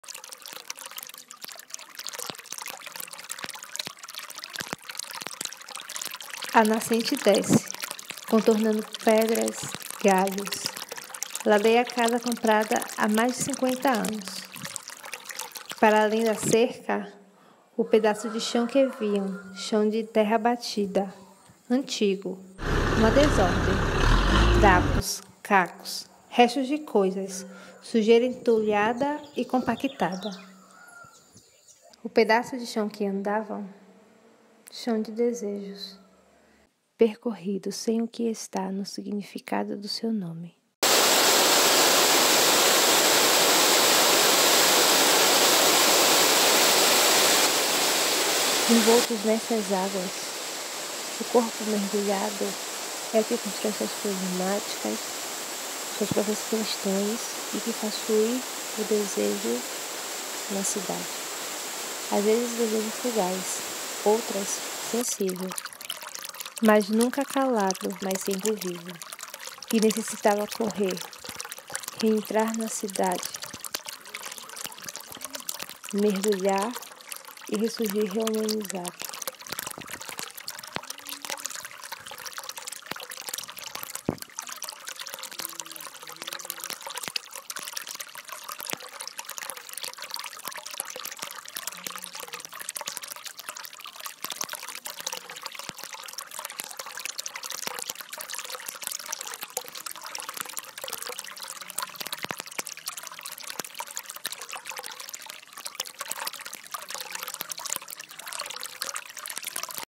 COMO SOM DE MUITAS ÁGUAS
Descrição:
Como som de muitas águas é uma obra totalmente independente e afetiva é a tradução de desejos para uma cidade; cidade onde nasci e a rua onde moro. O que tantas águas pode nos ensinar, para uma outra cidade? Quais desejos me são impostos por essas águas?